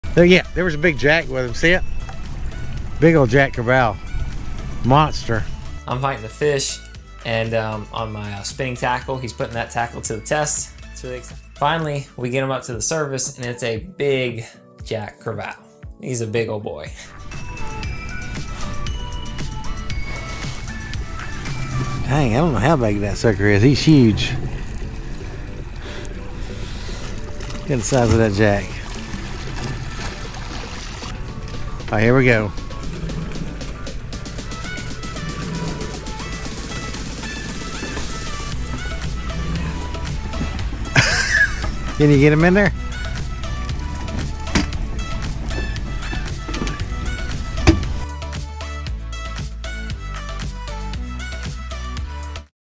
Huge Jack Crevalle that I Netted by Myself
While Cobia fishing I caught my personal best Jack Crevalle! This was the biggest Jack Crevalle we’ve ever seen!

Our Amazon Storefront
(Our Favorite Amazon Products in One Place)